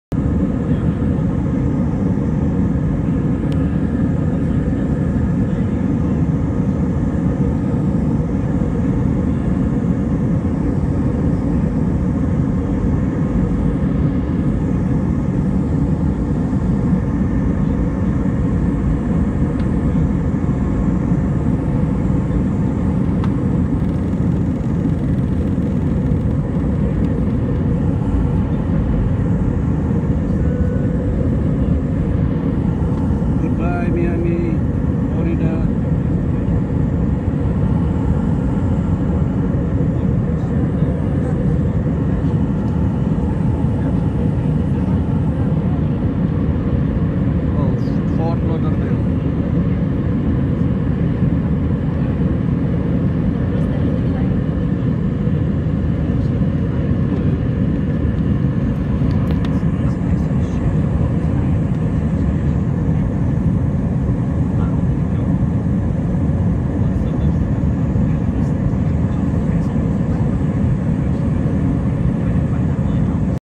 Take off from Fort Lauderdale Airport w/Jet blue..By Sammy Djr.
Take off from Fort Lauderdale, Florida with Jet Blue to Boston...Dec 2015...By Samuel "Sammy" Djamiraga.